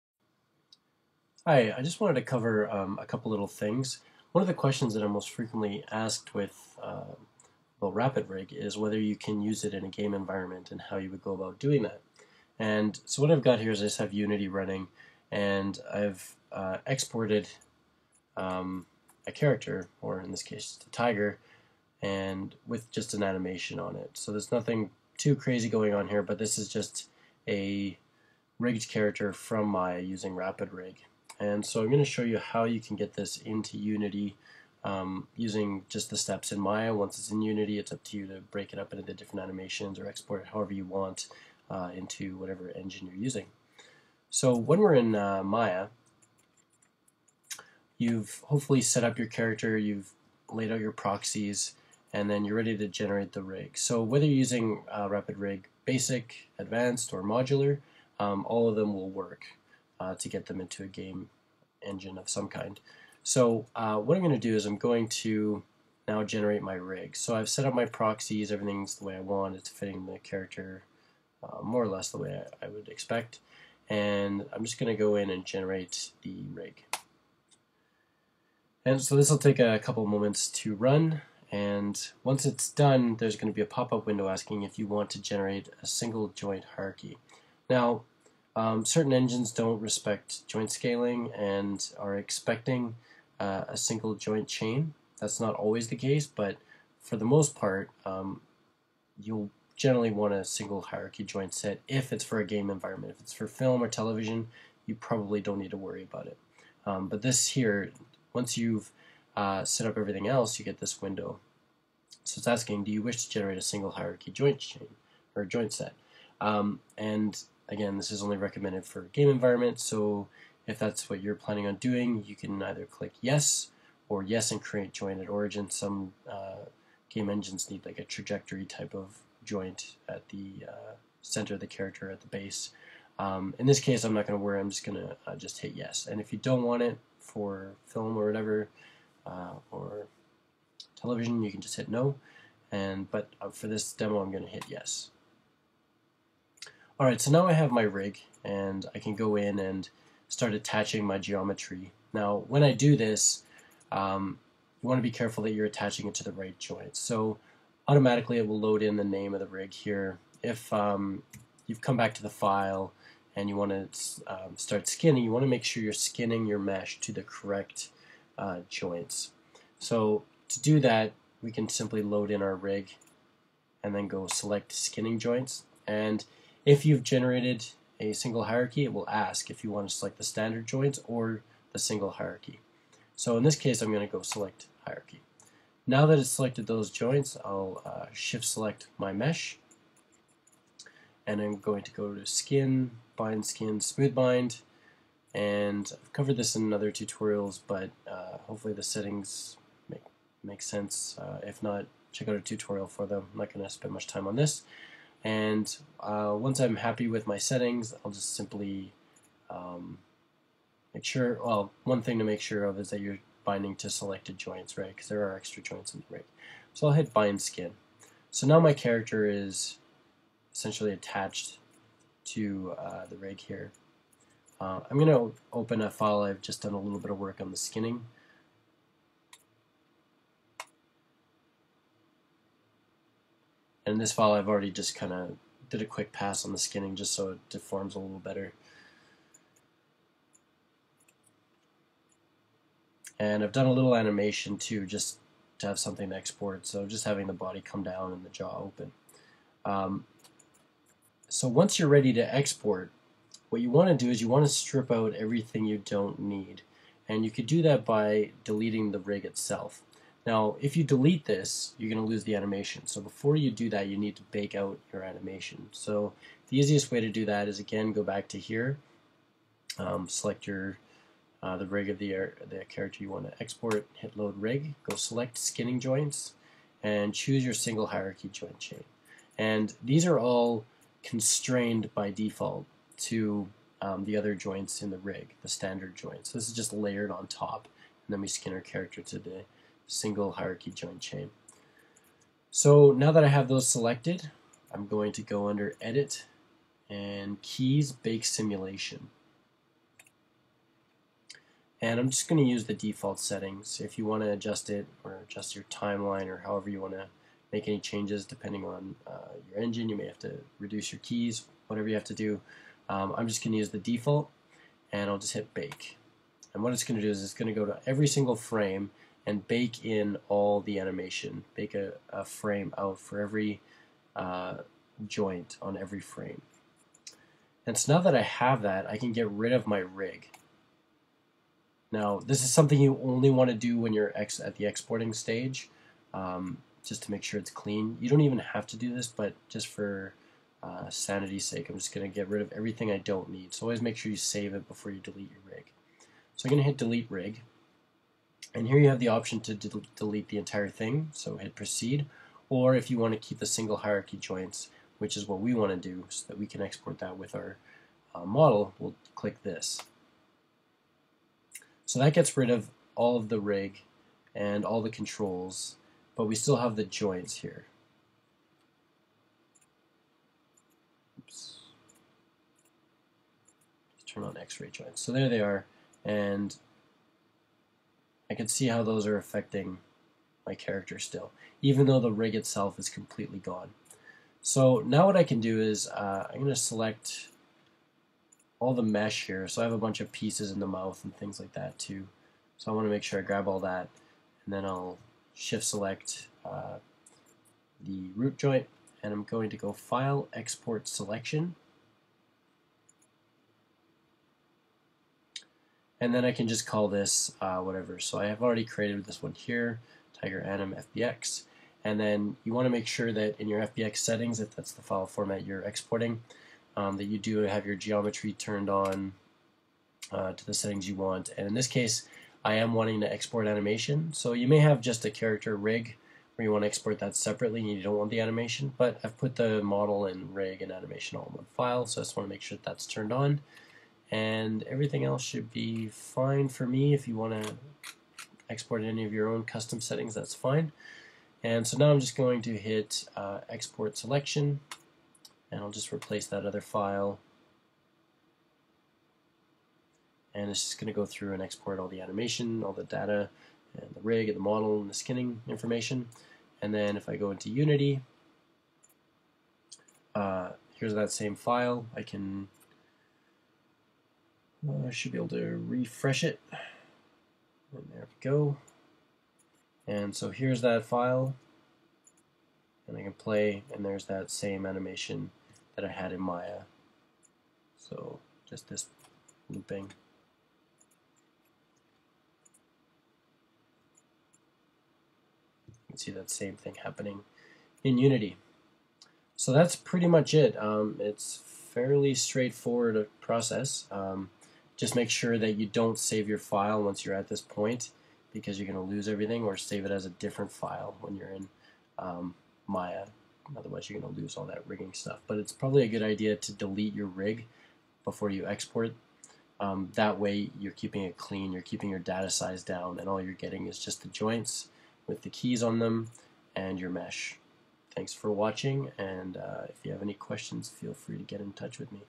Rapid Rig: Exporting For Games
In this brief tutorial, I cover how to set up and export a skinned mesh that uses Rapid Rig into a game engine.